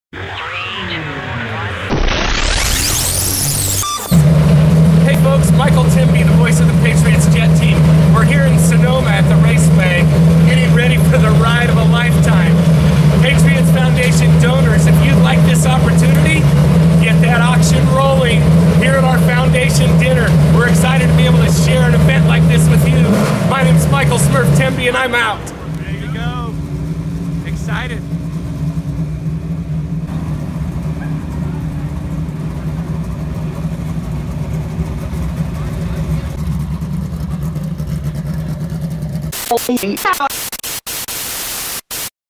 Behind the Scenes at Sonoma Indy Car with Mario Andretti
Michael “Smurf” Temby gets ready for the Indy Car Experience of a Lifetime.  Come to the Patriots Jet Team Gala and bid on the Chance to do the same